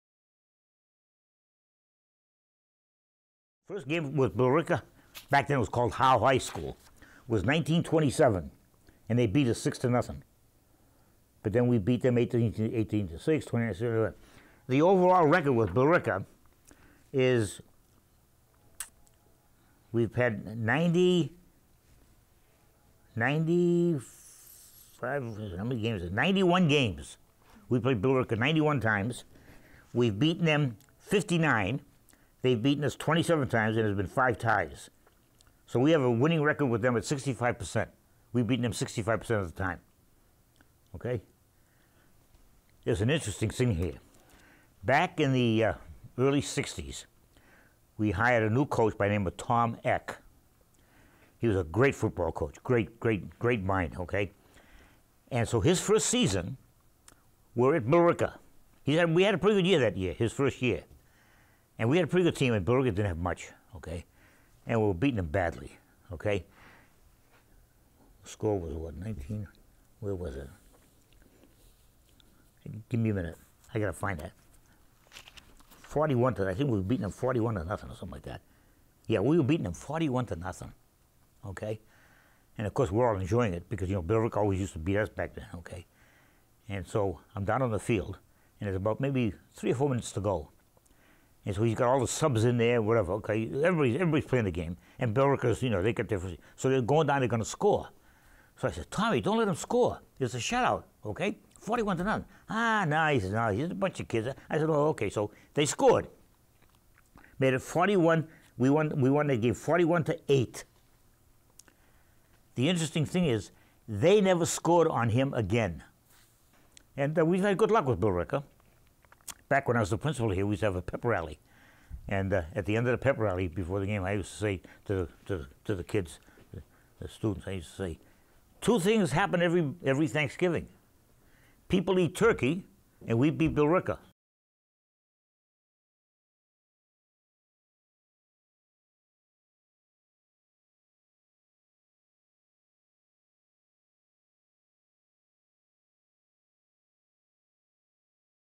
History of CHS Thanksgiving Football Rivalry with George Simonian
Principal Emeritus George Simonian talks with us about the long standing rivalry between the High Schools of Chelmsford and Billerica.  This is part of a larger interview which can be seen on the student produced 2018 Thanksgiving Day Pre-Game Show!